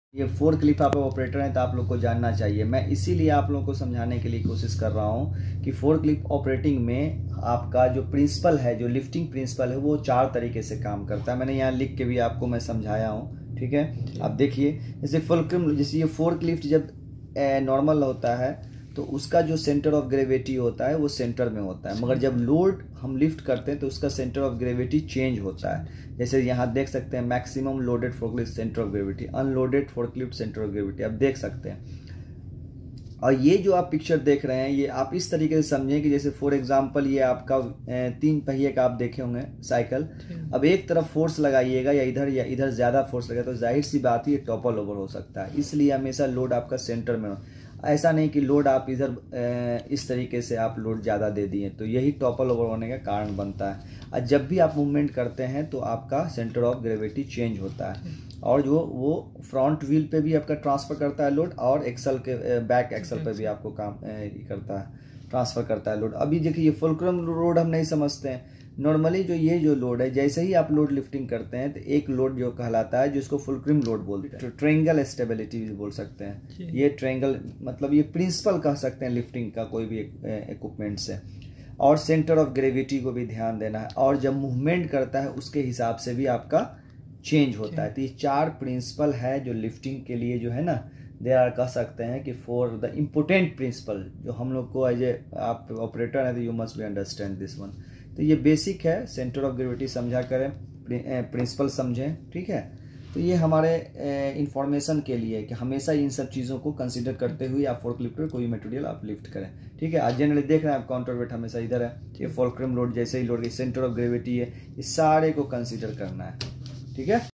Forklift Safety| Forklift load calculation| Safe working load| Forklift training| Equipment safety
Forklift safety calculation is very important for the operator and here is a calculation of what can be your safe working load and due to what needs and due to what actions our load can vary.. as operator lets find out safe lifting process. know factor which influence (@Healt &safety solution